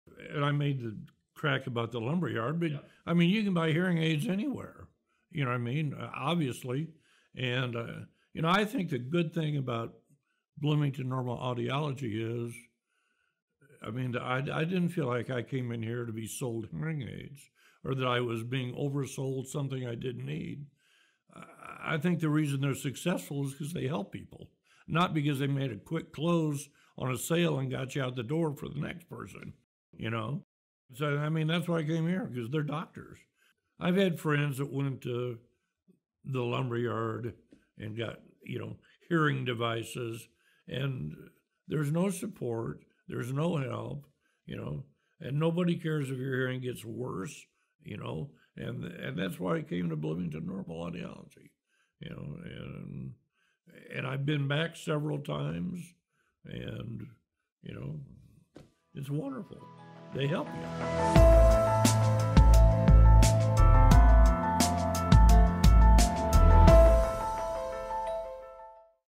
Doctors vs. Lumberyards | BNA - Hear My Story Soundbyte
“I think the reason they’re successful is because they help people.”

In this clip, Steve Peterson sums-up why he chose BNA.